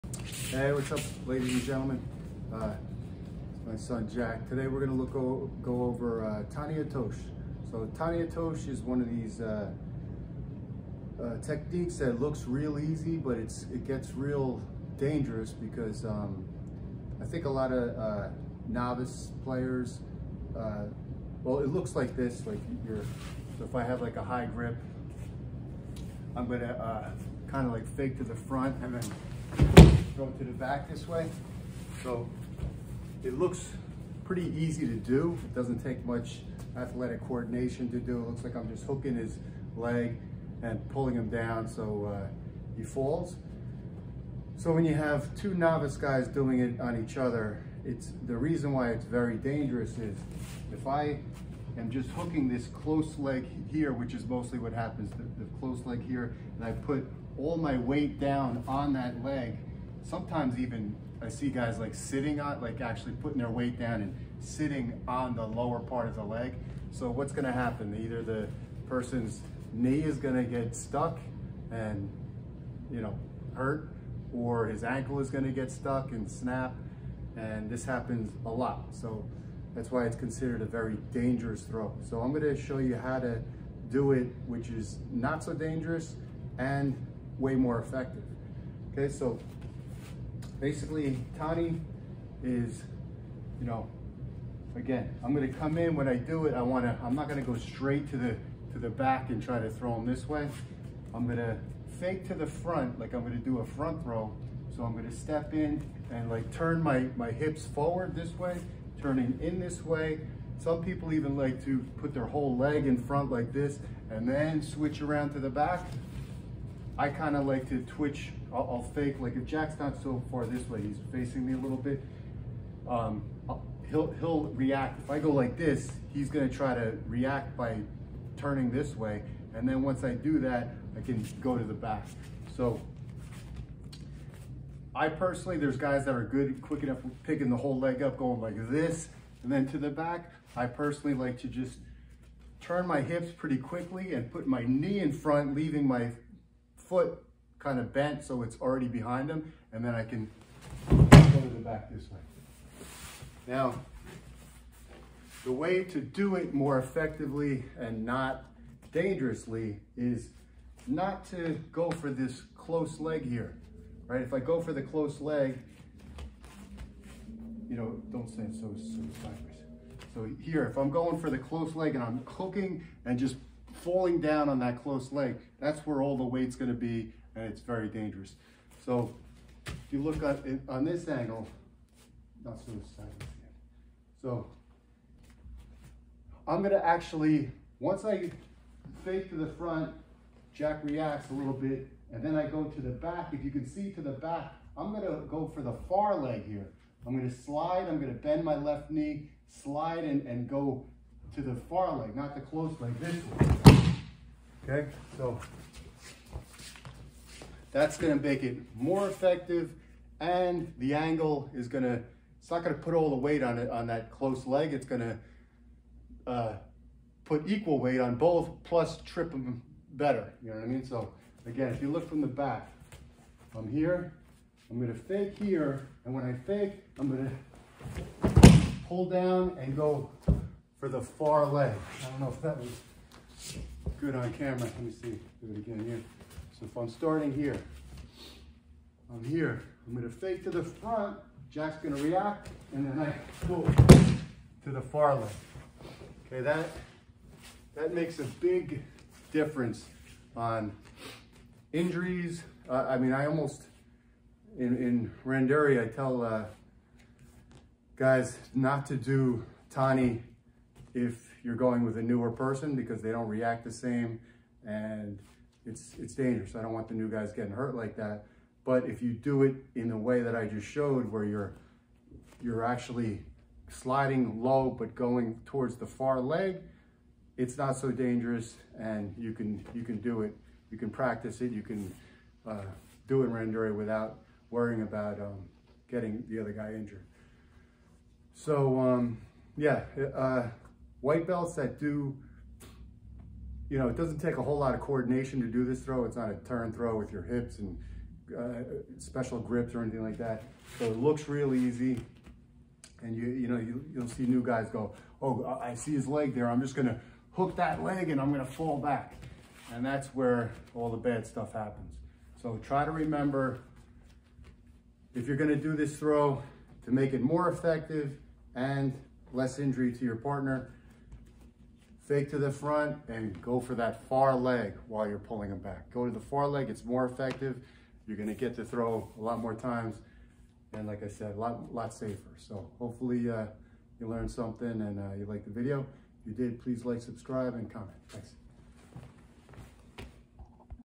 How to do Tani Otoshi more effectively without breaking uke’s leg
Judo Olympian at Cranford JKC showing “Tani Otoshi ”. Go here to see our website and schedule;